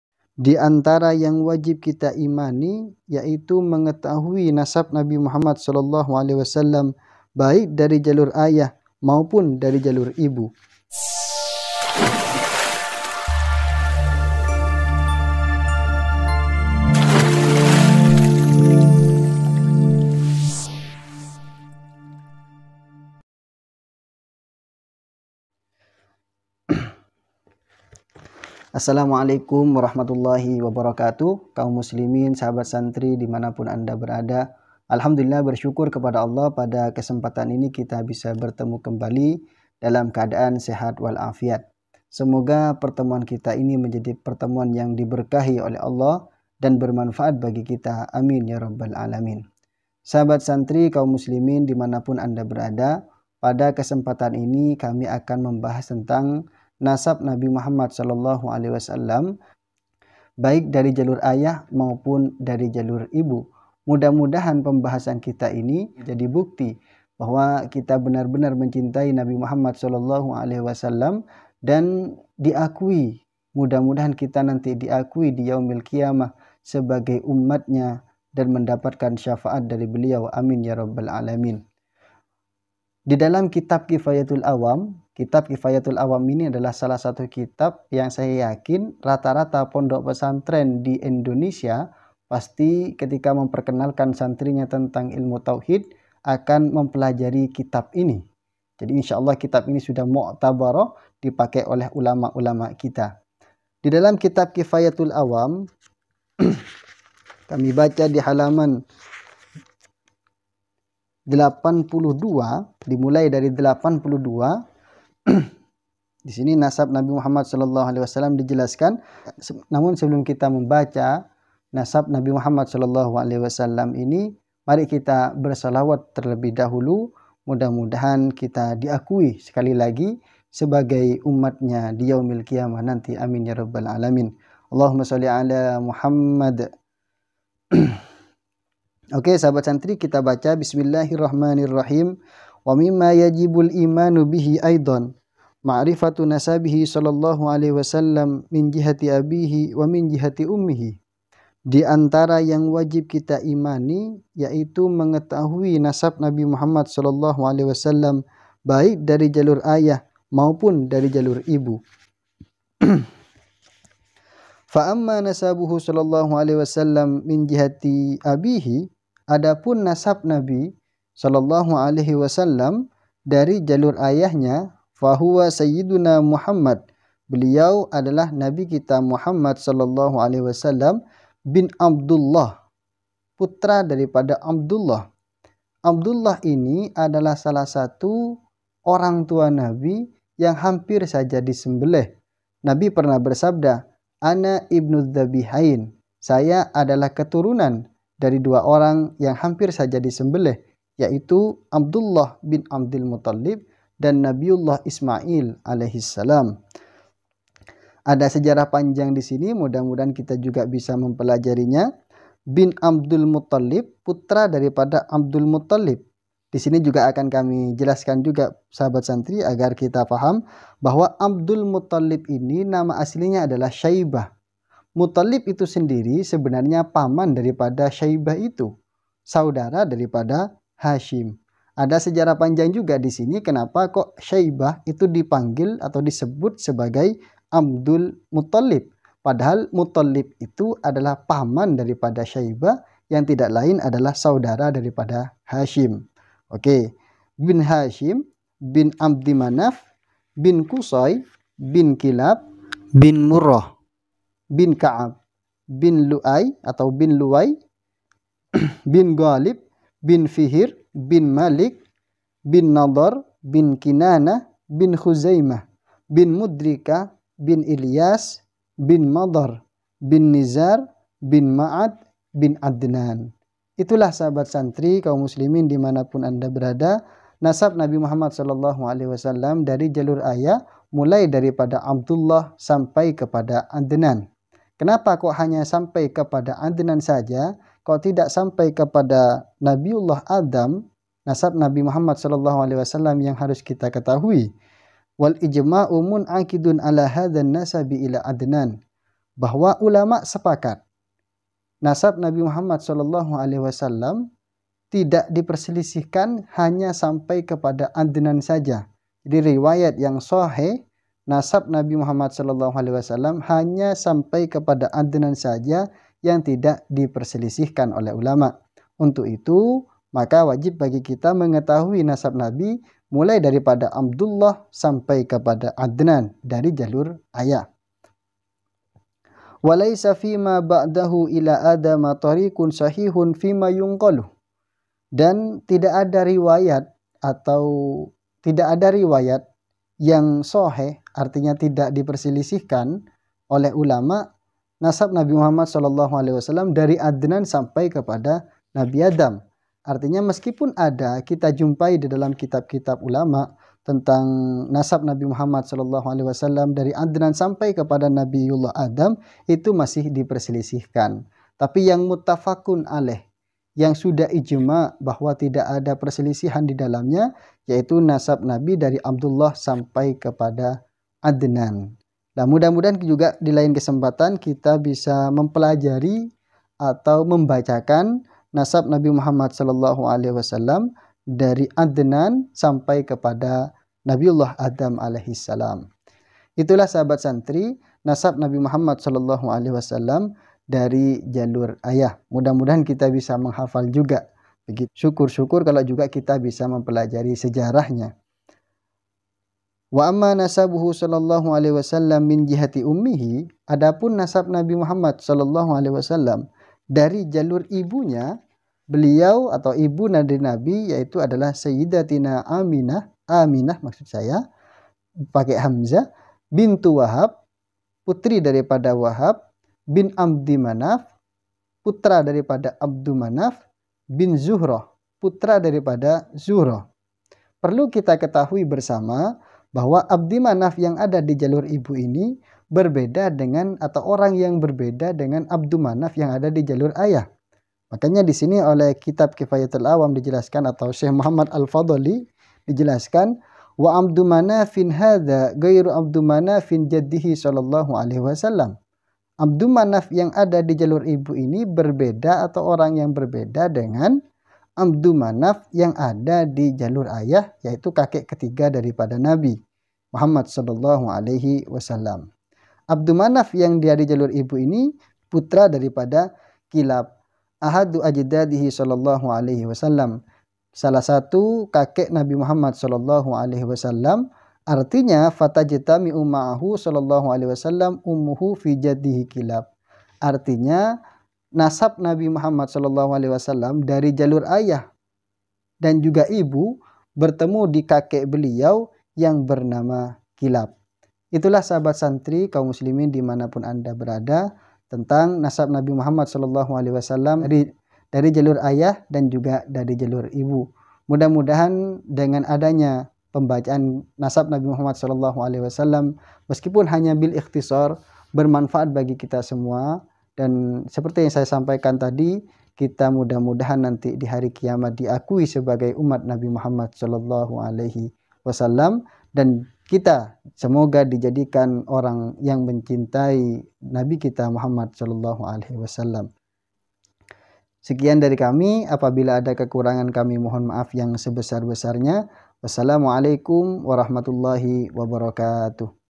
NASAB RASULULLAH SAW YG WAJIB DIKETAHUI || Cak Siroj
Nasab Nabi Muhammad SAW sampai kepda Adnan yg wajib kita ketahui, baik dari jalur ayah maupun jalur ibu

RASULULLAH SAW
@CAK SIROJ CHANNEL